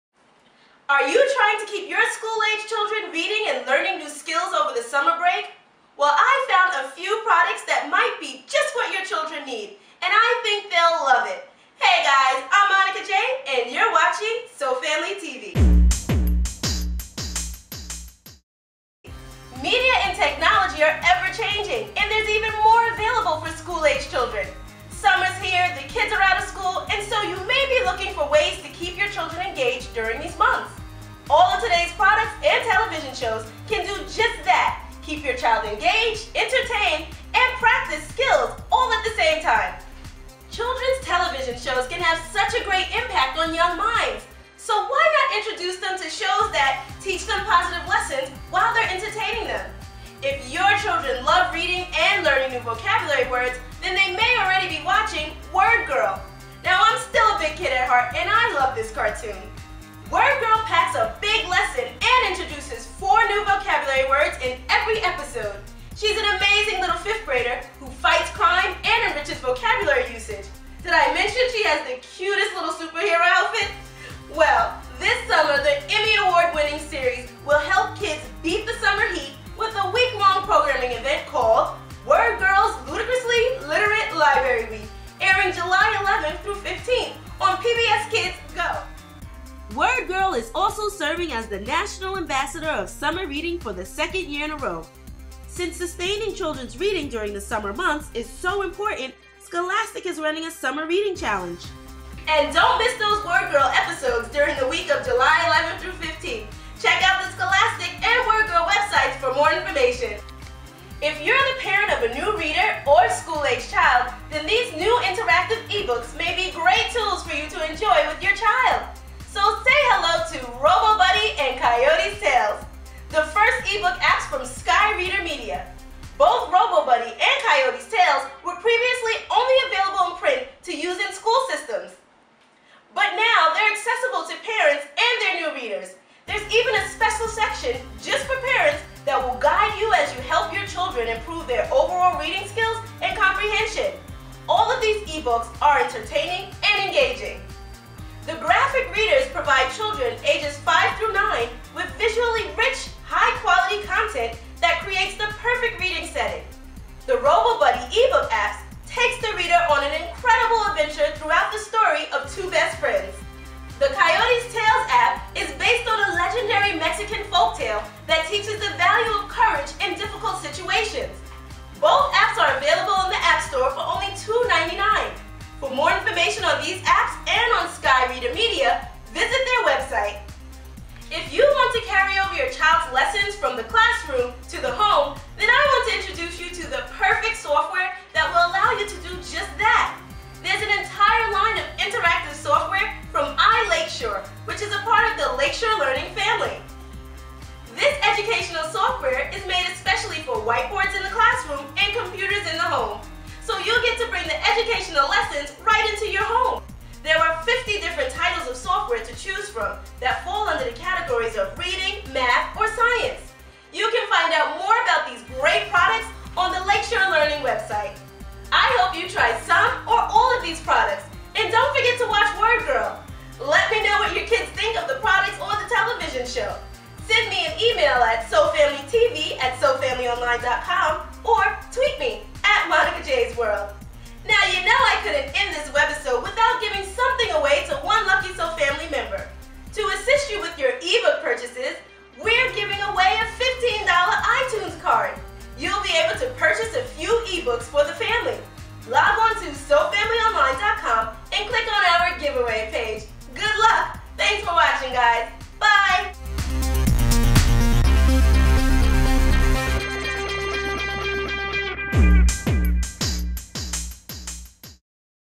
Technology for School Aged Kids - Word Girl, Robo Buddy, iLakeshore
Technology and media is ever-changing and now there is so much available for school-aged children.  From cd-roms to interactive ebook apps, your kids can continue learning and having fun all day.  We found some amazing products that your kids may love.  New ebook apps from Skyreader media, Robo Budyy and Coyotes Tales, will keep your kids engaged and excited to read.  An award winning cartoon, Word Girl, which teaches children new vocabulary every episode, 4 new vocabulary words to be exact.  There's also new interactive software to use on her home computer from ILakeshore, a division of Lakeshore Learning.  Your kids will have lots to do during their summer break and they will continue practicing educational skills while having fun.  for more information on these products.